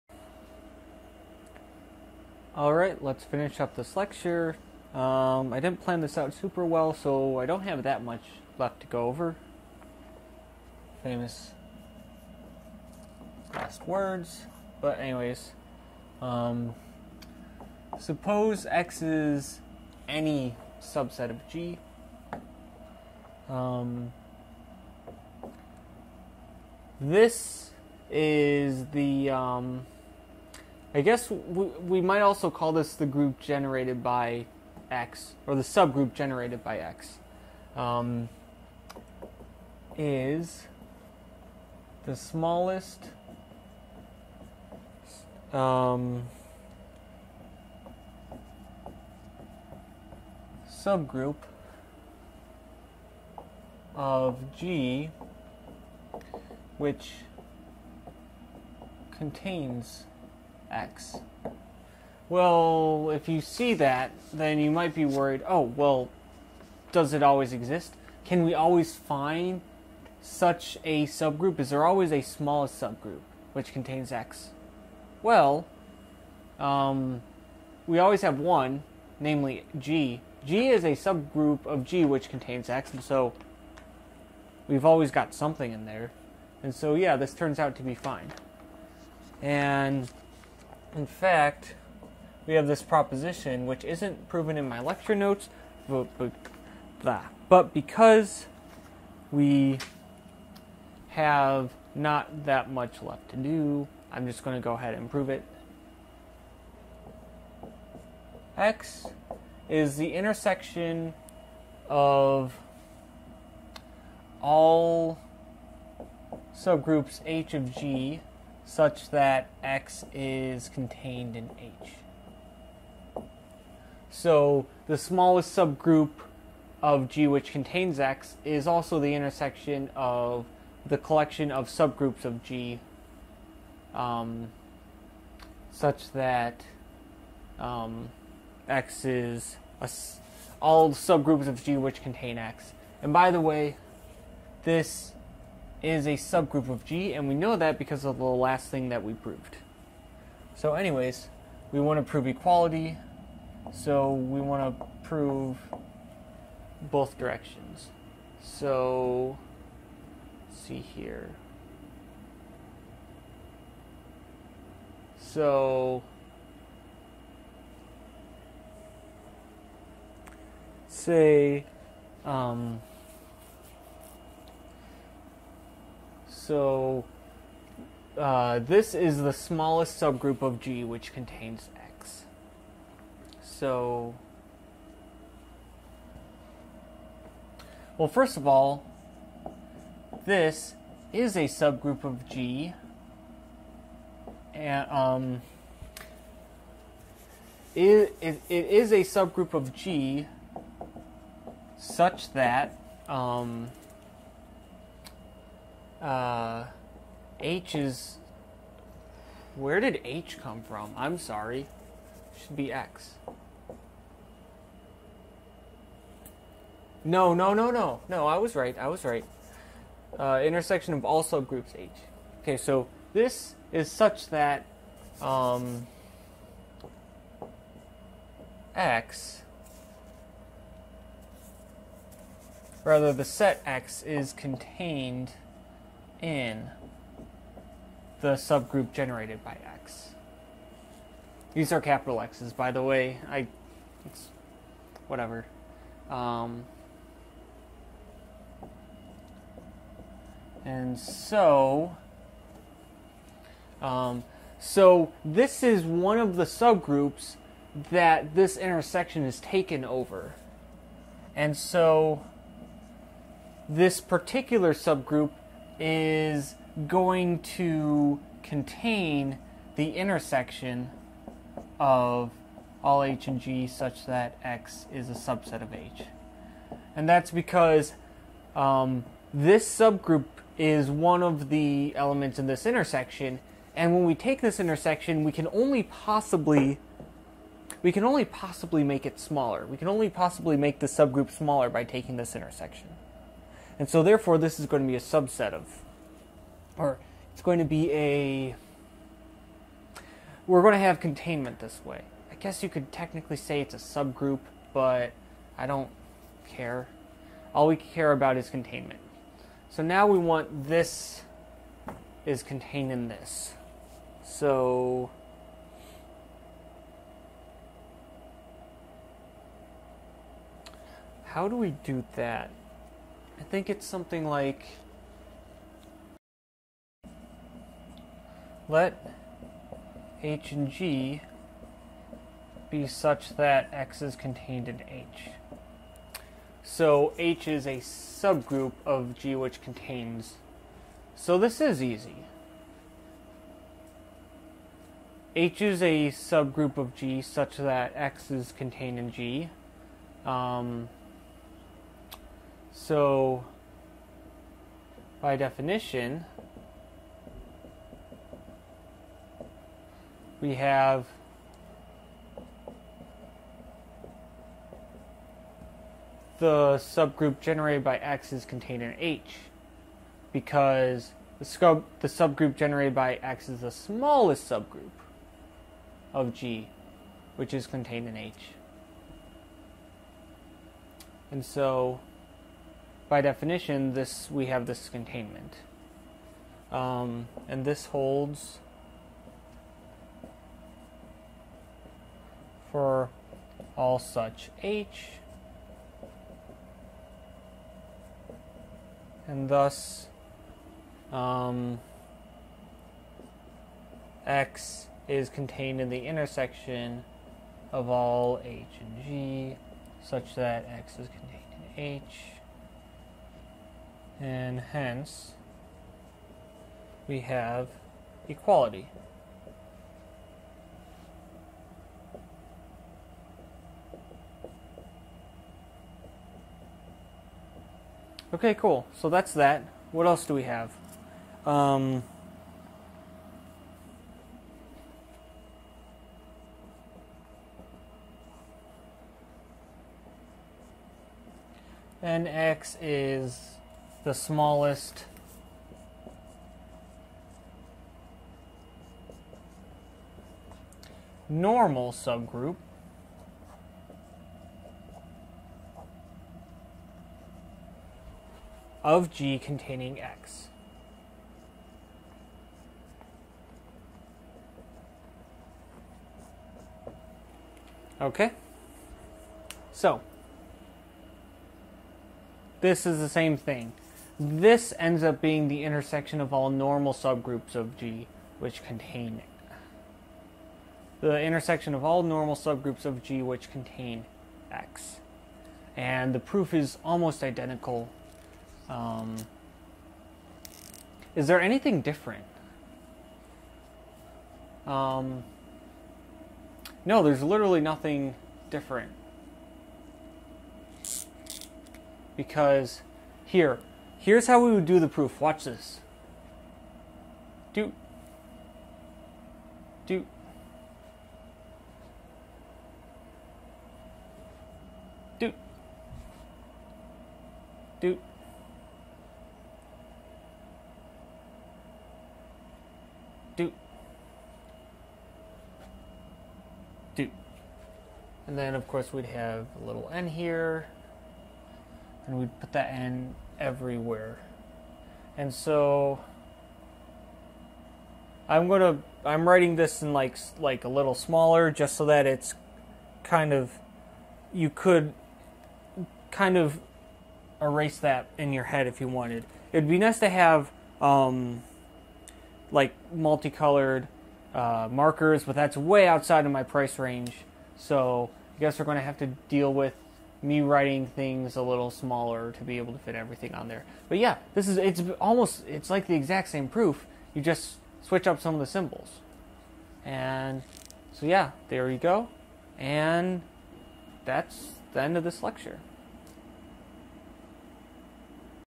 Group Lecture 2.4: Generated Subgroups
Part 4 of lecture 2 of my group theory lecture playlist. Topics discussed include subgroups generated by subsets of groups.